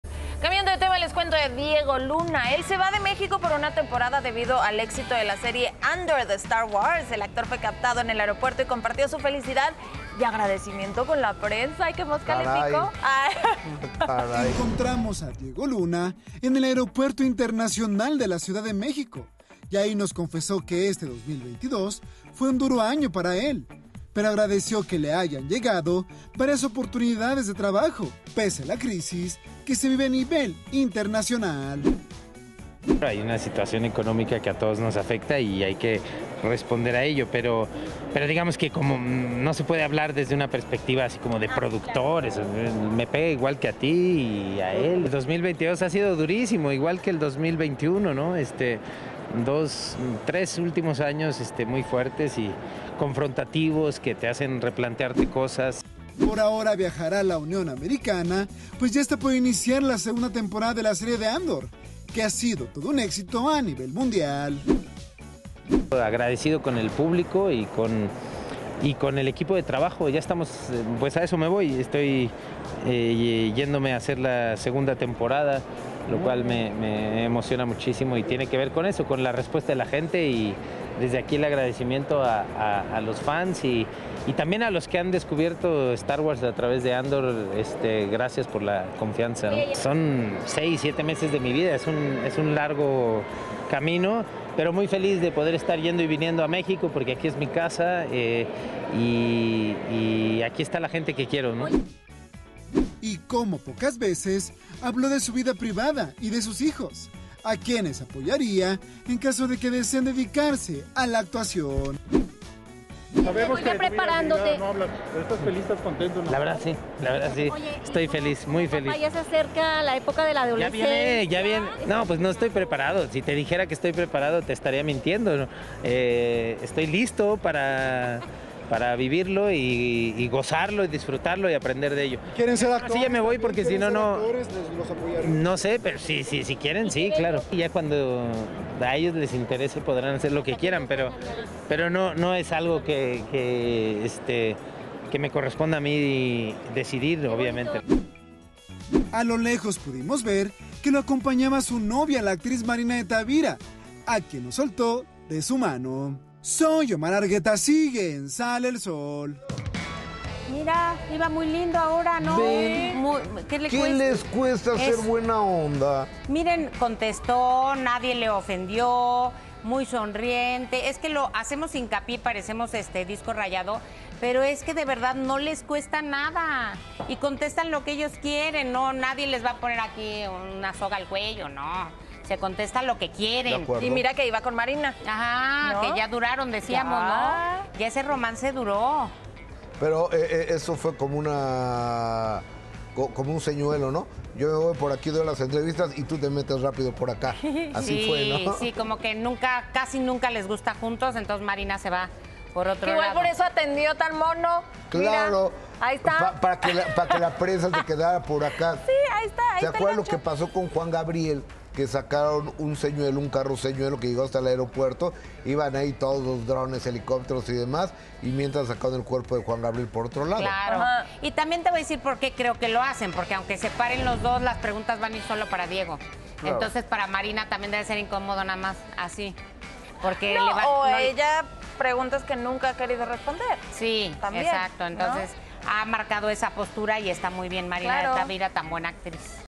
Diego Luna confirma segunda temporada de Andor | Sale el Sol
DiegoLuna sigue triunfando, ahora con la serie Andor, perteneciente al universo de StarWars y confirma una segunda temporada de ésta. Asimismo revela que apoyaría a sus hijos si decidieran emprender una carrera en la actuación.